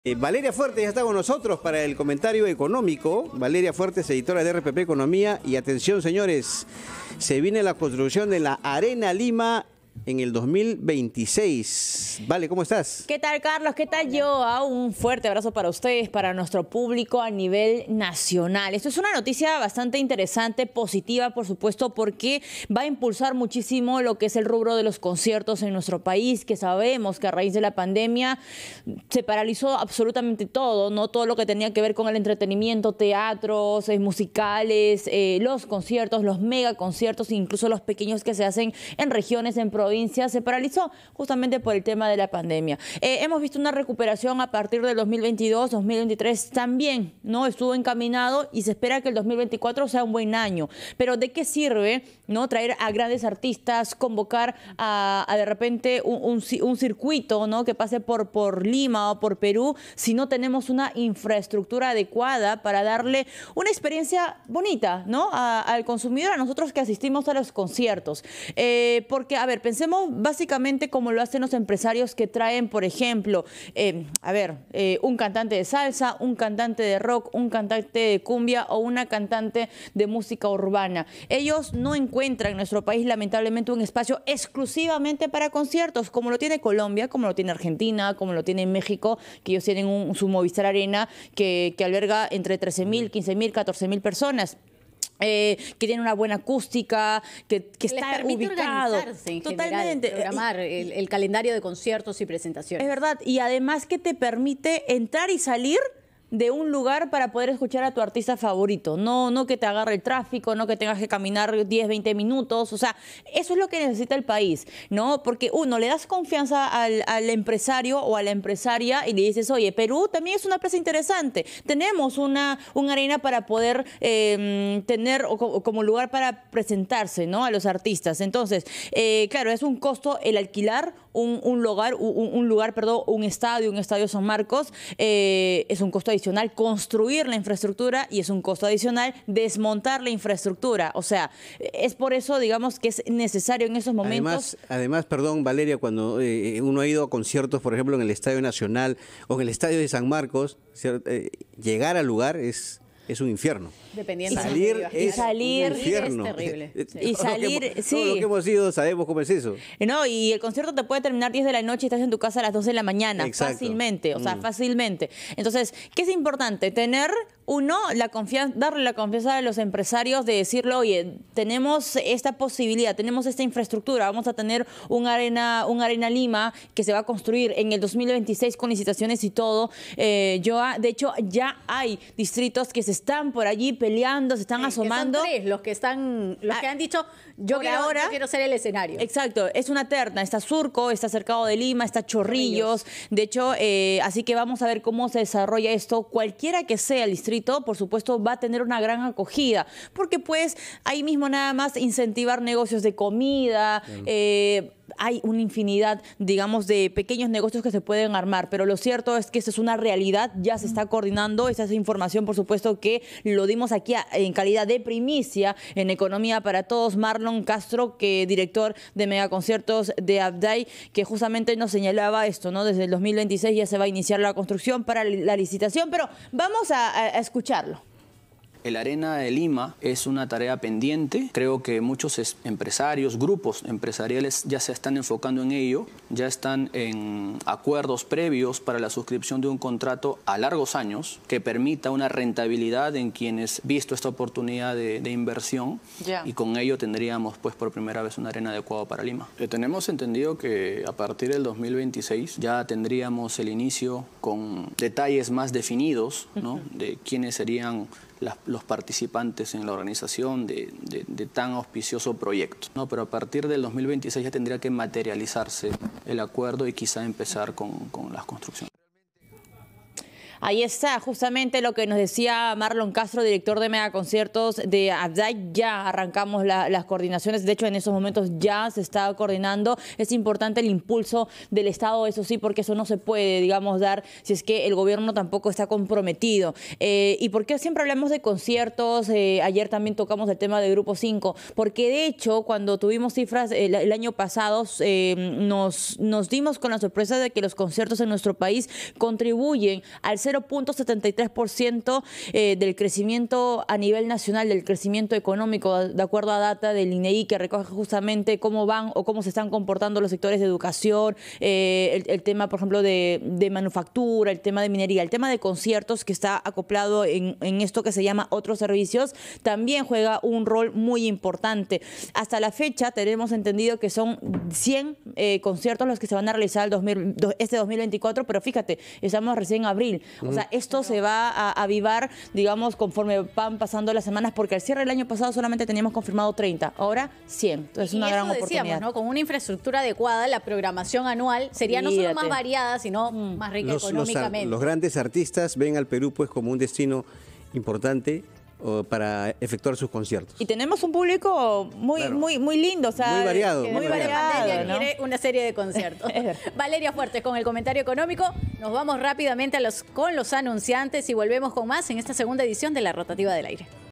Se empezaría la construcción del 'Arena Lima' en 2026
🎶 Organizadores de conciertos iniciaron coordinaciones para construir el 'Arena Lima' en 2026 para que la ciudad sea más atractiva para que se realicen conciertos. Surco, Cercado de Lima y Chorrillos ya postularon sus distritos para ser albergue de esta obra.